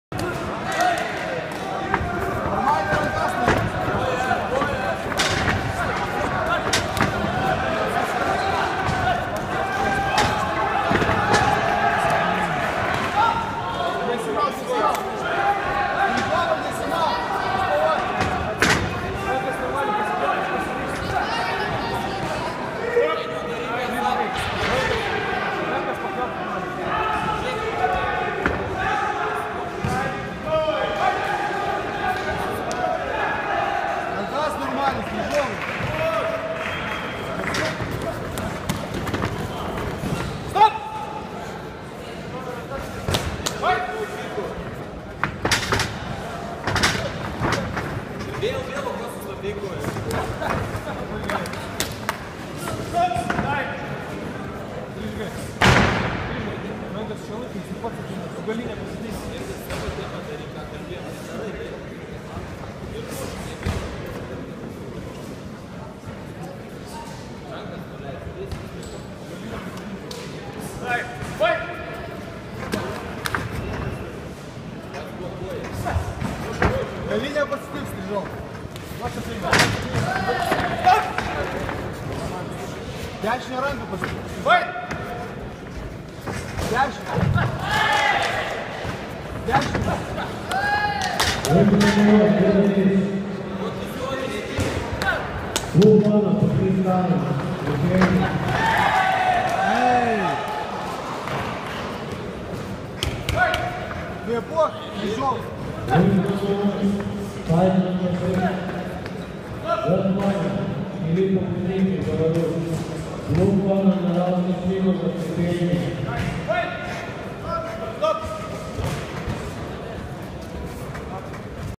Arturas Strizovas Vs Dainius Paskevius 1raund
European K-1 wak-1f championship:semifinals -48kg